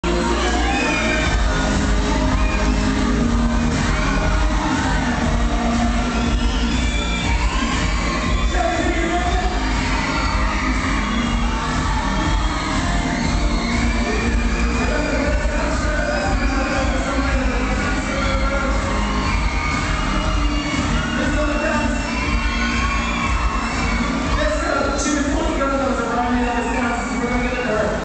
liam mckenna warming up crowd at jedward in university concert hall limerick
jedward concert in limerick